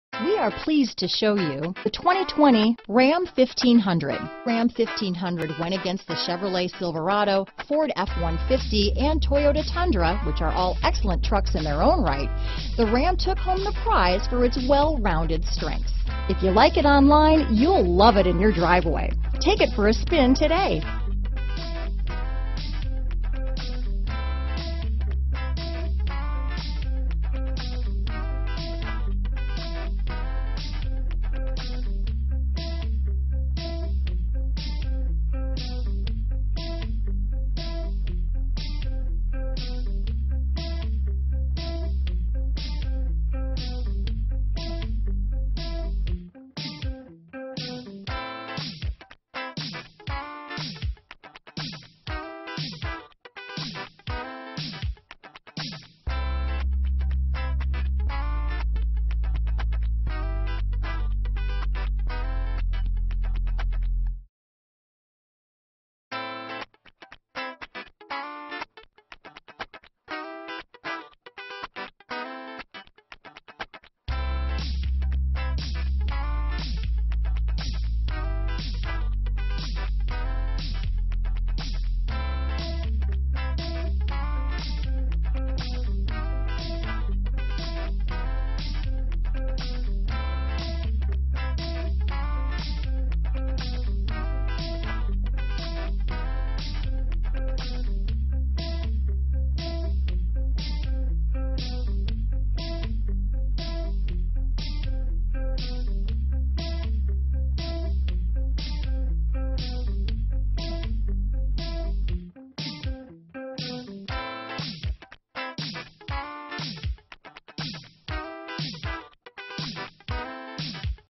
2020 Ram 1500 Granbury, Fort Worth, TX LN240015
New 2020 Ram 1500 available in Granbury, Texas at Mike Brown CDJR. Servicing the Fort Worth, TX area.
2020 Ram 1500 Lone Star - Stock#: LN240015 - VIN#: 1C6SRFFT3LN240015

This outstanding example of a 2020 Ram 1500 Lone Star is offered by Mike Brown Auto Group. Pull up in the vehicle and the valet will want to parked on the front row. This Ram 1500 Lone Star is the vehicle others dream to own. Dont miss your chance to make it your new ride. This 4WD-equipped vehicle will handle majestically on any terrain and in any weather condition your may find yourself in. The benefits of driving an Four Wheel Drive vehicle such as this Ram1500 Lone Star include superior acceleration improved steering and increased traction and stability. Just what youve been looking for. With quality in mind this vehicle is the perfect addition to take home. The Ram 1500 Lone Star will provide you with everything you have always wanted in a car -- Quality Reliability and Character.